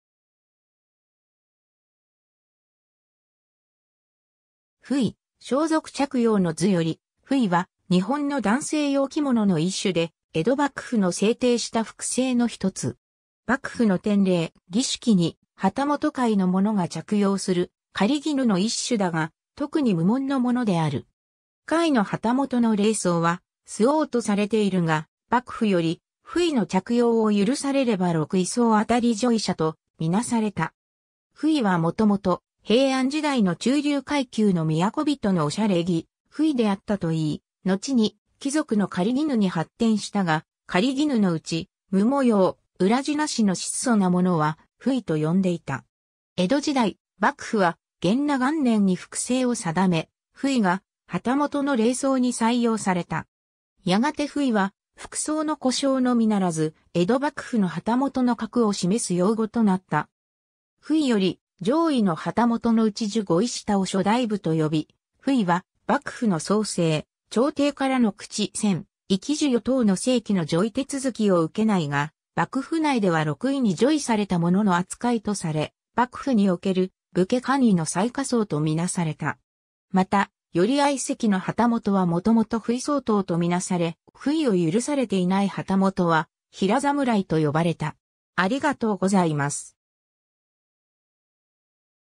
布衣
布衣、『装束着用之図』（国立国会図書館蔵）より 布衣（ほい）は、日本の男性用着物の一種で、江戸幕府の制定した服制の1つ。
幕府の典礼・儀式に旗本下位の者が着用する狩衣の一種だが、特に無紋（紋様・地紋の無い生地）のものである。
下位の旗本（すなわち御目見以上）の礼装は素襖とされているが、幕府より布衣の着用を許されれば六位相当叙位者と見なされた。
 布衣は元々、平安時代の中流階級の都人のお洒落着・布衣（ほうい）であったといい、のちに貴族の狩衣に発展したが、狩衣のうち無模様・裏地なしの質素なものは布衣と呼んでいた。
江戸時代、幕府は元和元年（1615年）に服制を定め、布衣が旗本の礼装に採用された。
やがて布衣は服装の呼称のみならず江戸幕府の旗本の格を示す用語となった。
 布衣より上位の旗本（従四位上～従五位下）のうち従五位下を諸大夫（しょだいぶ）と呼び、布衣は幕府の奏請、朝廷からの口宣・位記授与等の正規の叙位手続きを受けないが、幕府内では六位に叙位された者の扱いとされ、幕府における武家官位の最下層とみなされた。
また、寄合席（三千石以上）の旗本はもともと布衣相当とみなされ、布衣を許されていない旗本は平士（へいし）とよばれた。